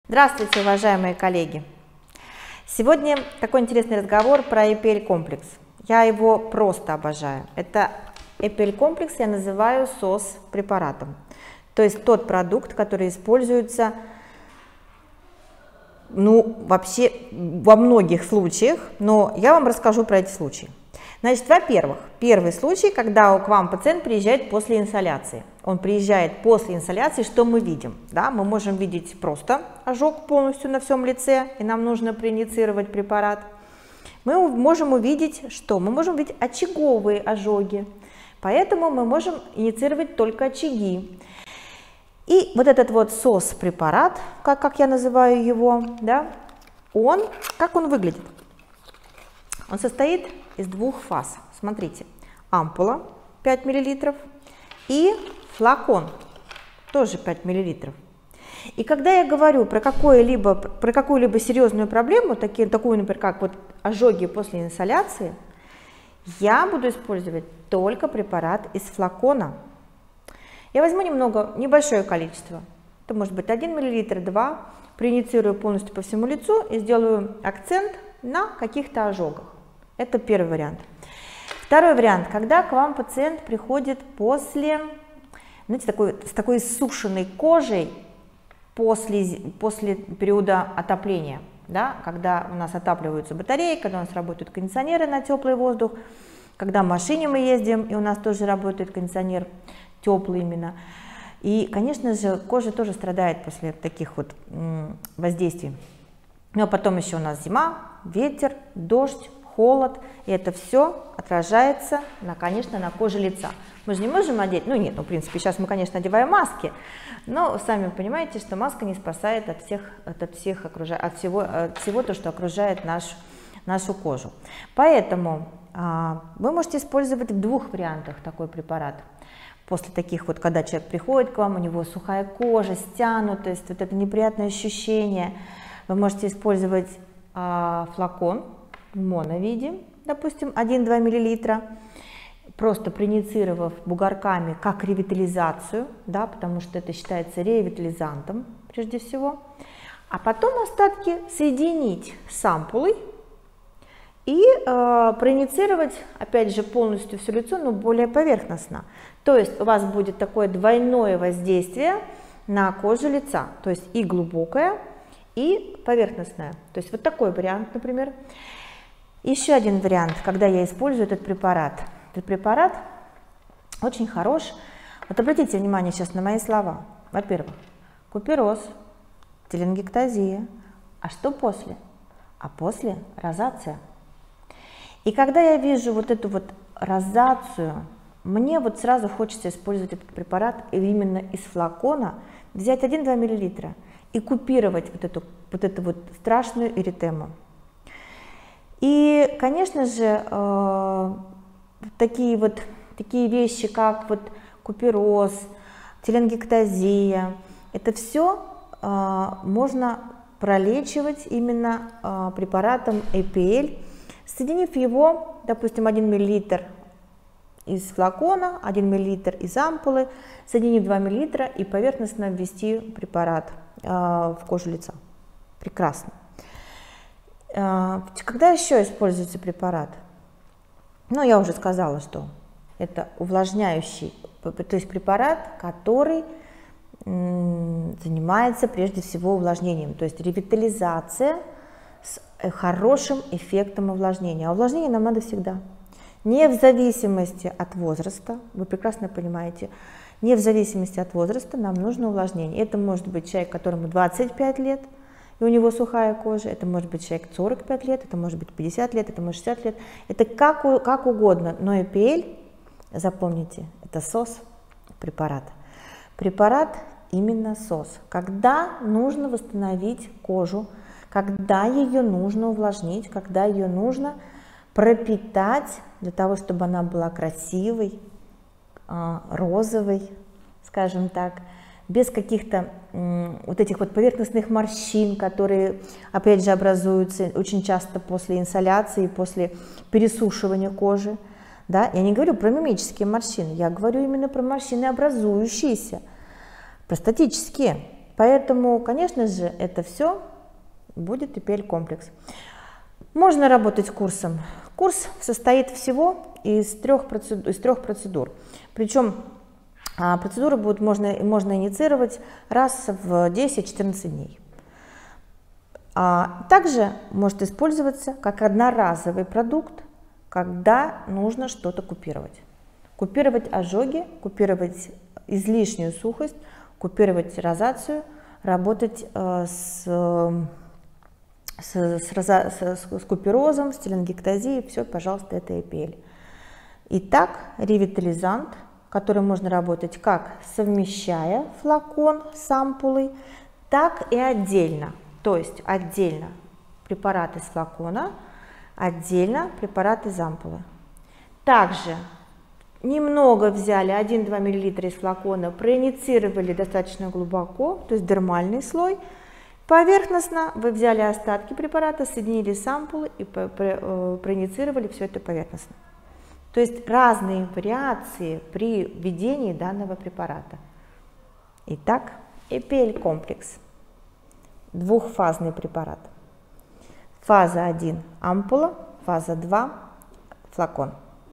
Ревитализант DERLI APL complex – препарат SOS для косметолога и пациентов
Используется для экстренной помощи при состояниях кожи после активной инсоляции, солнечных ожогов, излишней сухости, «стянутости», также в терапии купероза, телеангиэктазии, розацеа.

DERLI APL complex – ревитализант, обогащенный пептидами растительного происхождения. Состоит из двух фаз: первая фаза находится во флаконе, вторая – в ампуле.

Для экстренной помощи после инсоляции, солнечных ожогов достаточно провести процедуру или курс (в зависимости от состояния кожи) биоревитализации содержимым фазы №1 (во флаконе).

Для коррекции состояния иссушенной кожи (после «сезона батарей», чередования ветра, дождя, холода), когда кожа обезвожена, появляются поверхностные статические морщинки, беспокоит чувство «стянутости», рекомендуется инъецировать 1-2 мл фазы №1 (во флаконе) бугорковой техникой, остатки смешать с фазой №2 (в ампуле) и инъецировать любой поверхностной техникой.

Курс: 3 процедуры с интервалом 10-14 дней.
Экстренная помощь: однократно.

При куперозе, телеангиэктазии рекомендуется смешать по 1 мл фазы №1 и №2, инъецировать поверхностно.

Эти и другие советы по использованию DERLI APL complex рассказывает врач-косметолог MESOPROFF Юсупова Олеся.